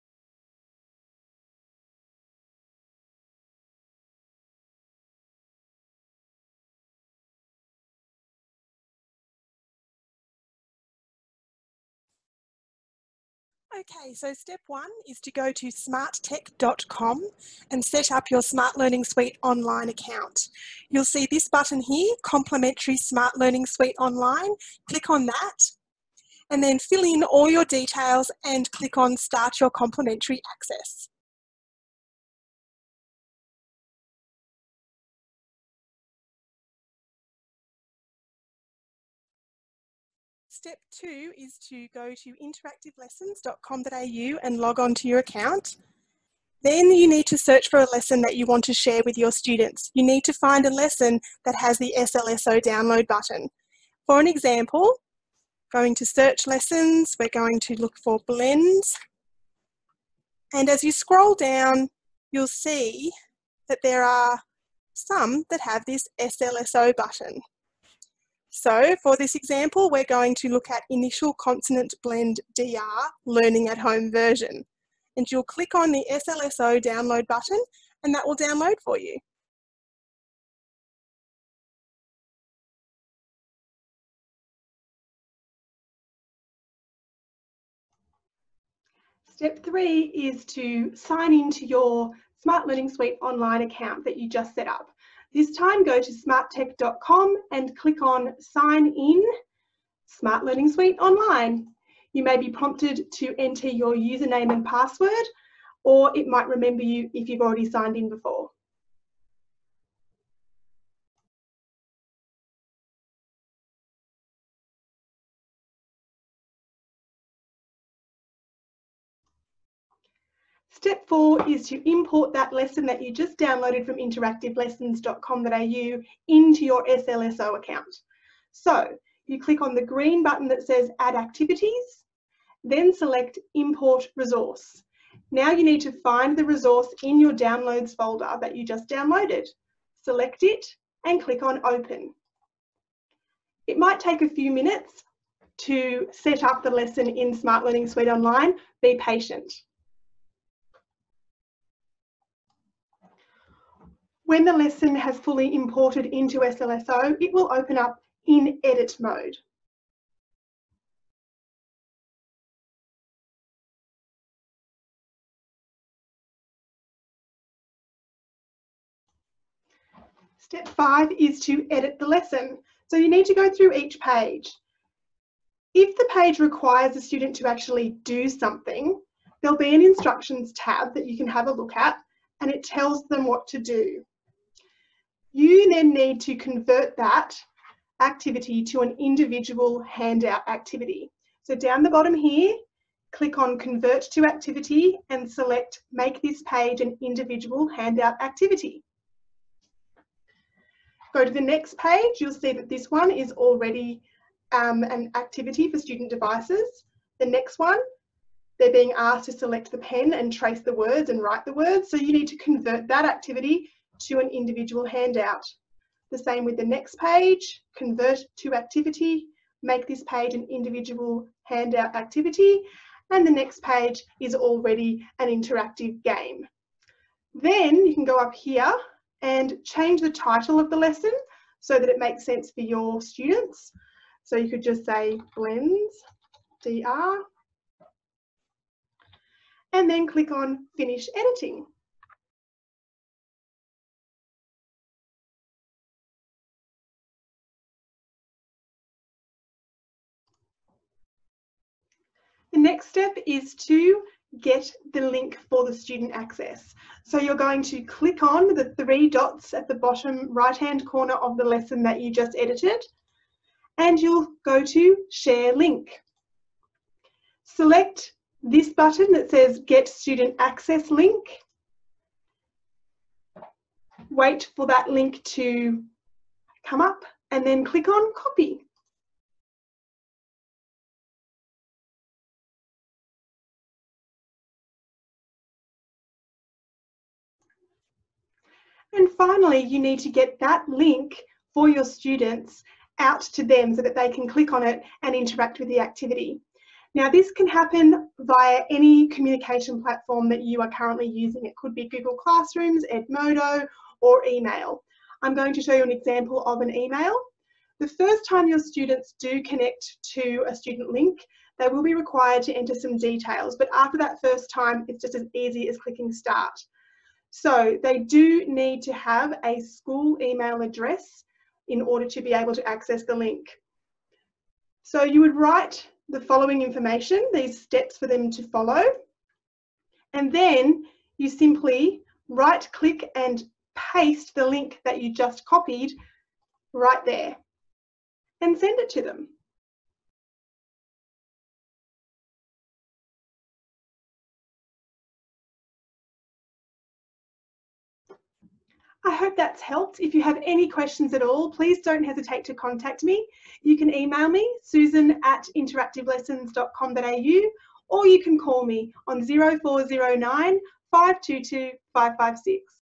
Learning at Home Interactive Lessons with SLSO
Susan has re-created lots of her IWB lessons from interactivelessons.com.au and has specifically designed them for independent completion at home on any internet-connected student device via SMART Learning Suite Online (SLSO). This video show you exactly how to import the resources, edit them and send the student link out to your students at home.